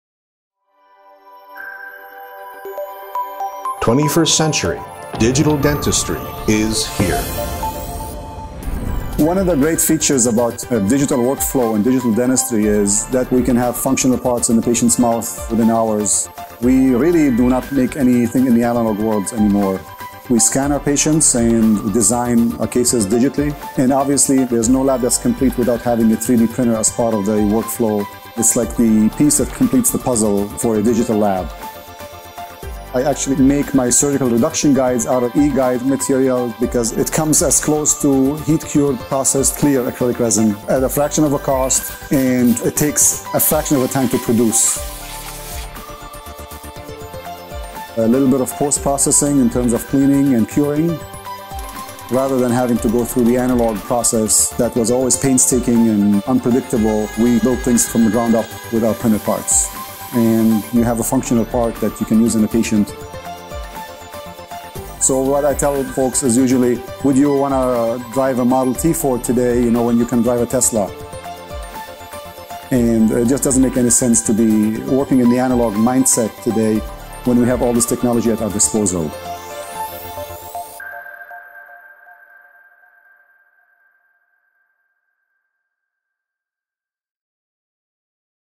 E-Guide | 3D Printed Surgical Drill Guides
EnvisionTEC’s E-Guide is a biocompatible certified Class I material, developed for the production of high precision surgical drill guides for use in implant surgery. The results produced by combining E-Guide with EnvisionTEC technology are superior to traditional methods of manufacturing implant placement guides.

High resolution 3D printing allows for precisely targeted placement of implants during surgery. Drill sleeves may be inserted directly after printing, with excellent fit. The material is resistant to disinfectants and can be sterilised using gamma rays and autoclave without affecting dimensional stability.

The cost of high quality drill guides can be dramatically lower when produced using E-Guide on an EnvisionTEC 3D printer in comparison to previous machining methods, making EnvisionTEC the clear choice for any digital dental laboratory.

whenyousmile.us